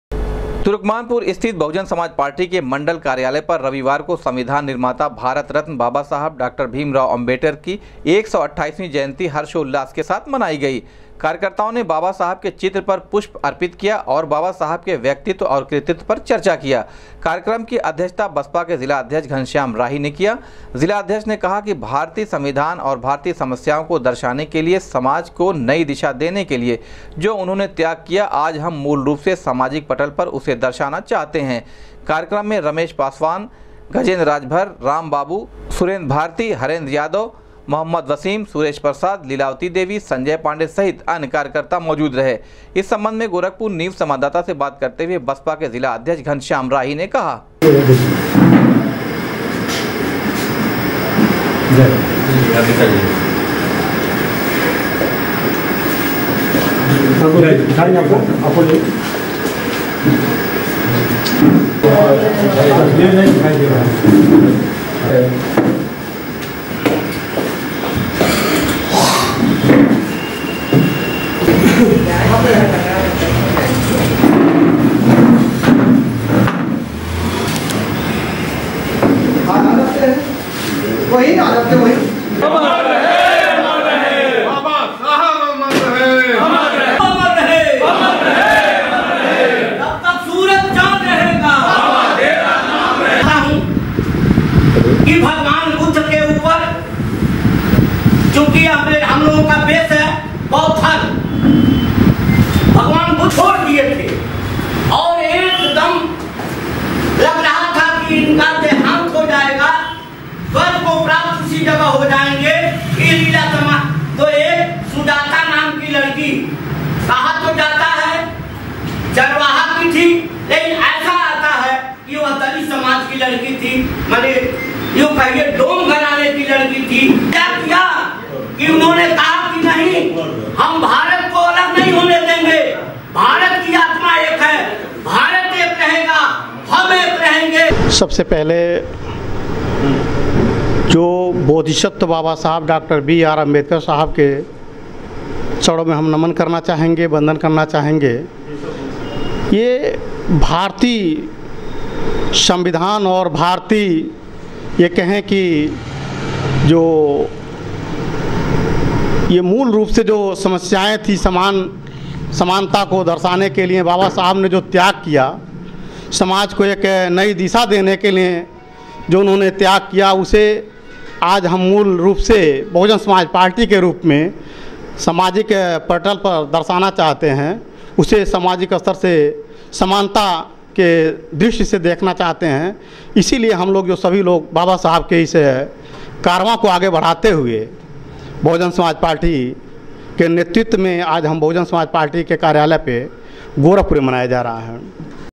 हर्षोल्लास से मनाया बसपा ने बाबा साहब का 128वां जन्मदिवस, कहा बाबा साहब के कारवां को
हर्षोल्लास से मनाया बसपा ने बाबा साहब का 128वां जन्मदिवस, कहा बाबा साहब के कारवां को आगे बढ़ाने के लिए मना रहे हैं बाबा साहब का जन्म उत्सव